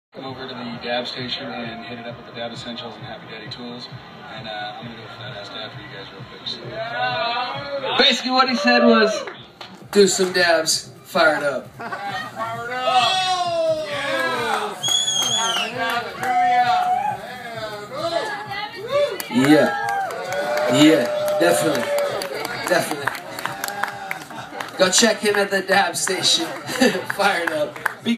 PNW Harvest Fest Dab Demonstration-DABratory
Zzyzx giving a Dab demonstration at the PNW Harvest Fest Cannabis Cup September 2012